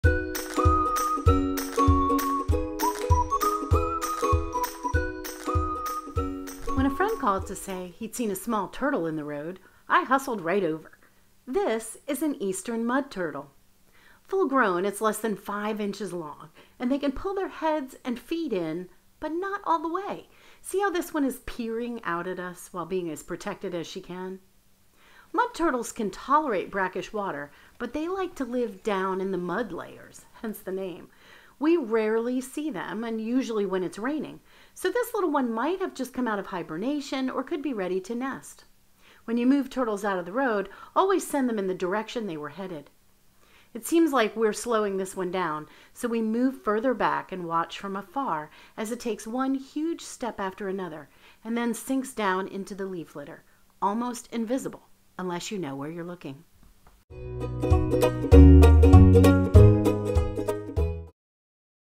Mud Turtle
When a friend called to say he’d seen a small turtle in the road, I hustled right over.  This is an eastern mud turtle.  Full grown, it’s less than 5 inches.  They can pull their heads and feet in, but not all the way: see how this one is peering out at us while being as protected as she can? Mud turtles can tolerate brackish water, but they like to live down in the mud layers. We rarely see them, usually when it's raining.  This little one might have just come out of hibernation or could be ready to nest. When you move turtles out of the road, send them in the direction they were headed.  We watch from afar as it takes one huge step after another, and then sinks down into the leaf litter, almost invisible unless you know where you’re looking.